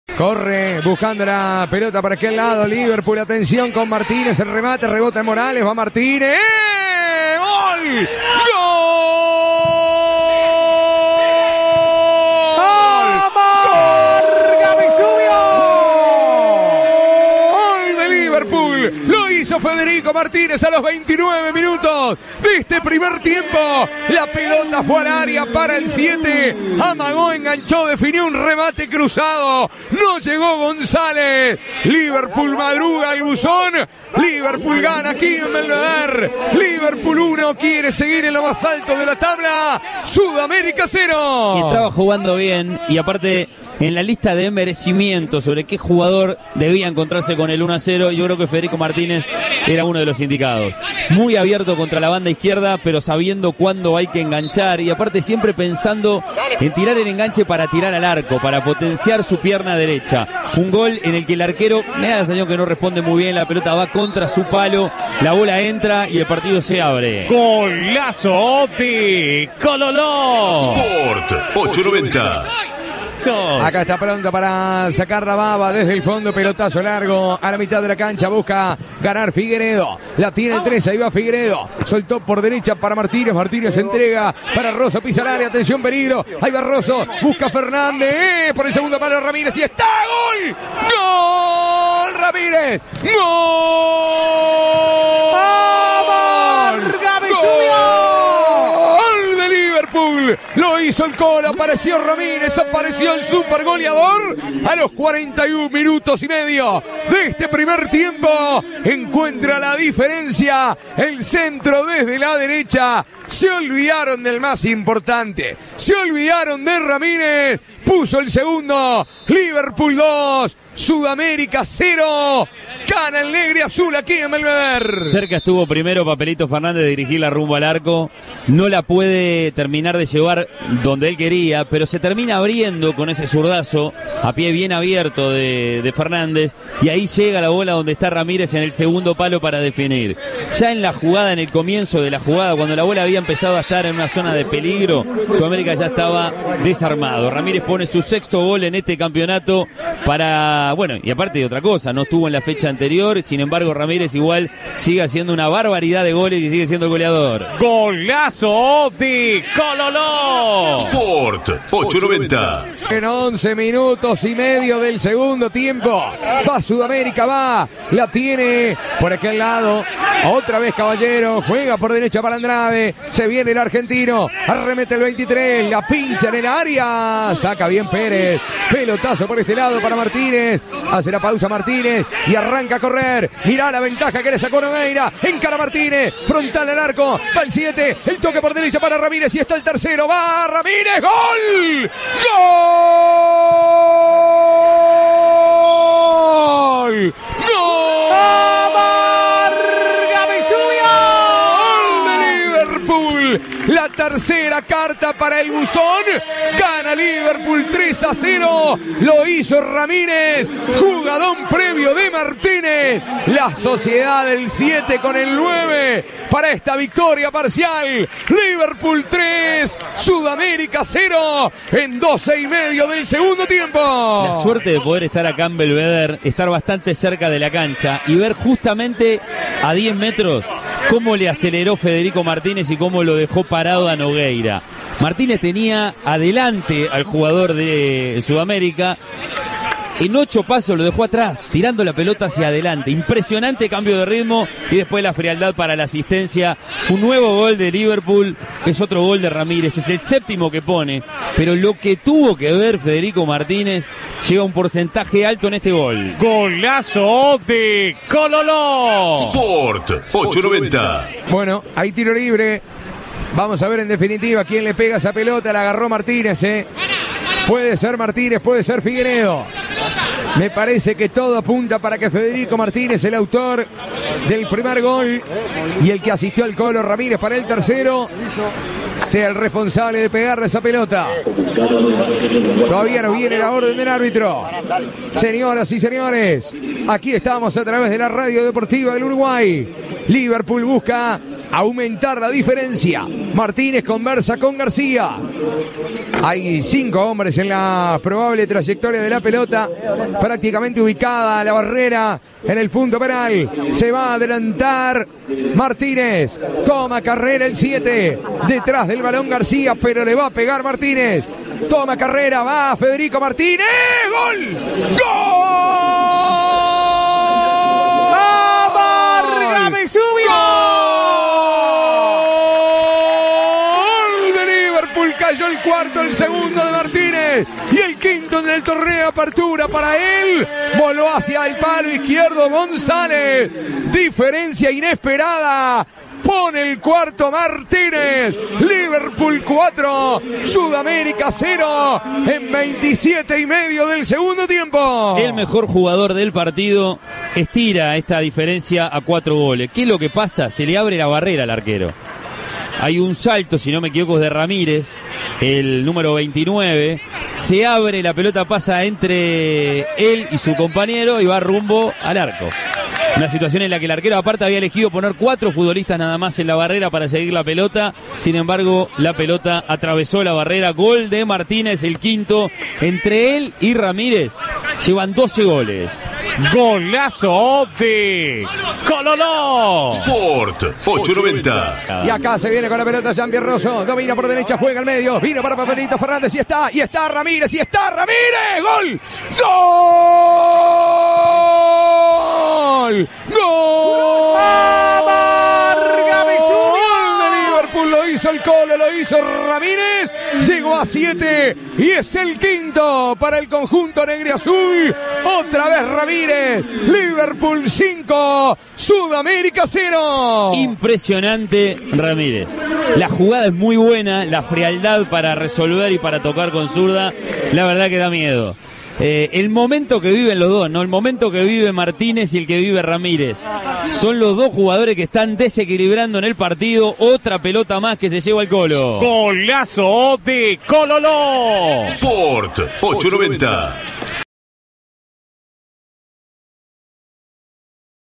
Liverpool 5 Sud América 0 - Apertura 2021 (por Diego Jokas)
Los goles del partido, por Diego Jokas y Martín Charquero para CX 18 Radio Sport 890 AM.